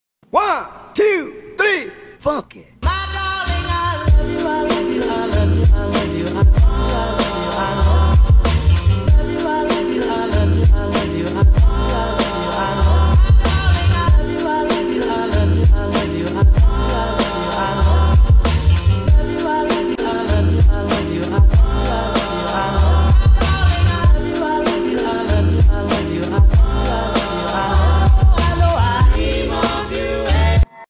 I Love You // meme(UnS)
Finally an UnaturalSupers animation w/ Sam the redhead obvi, And Ayano the other one who is RE4LLY H4RD to draw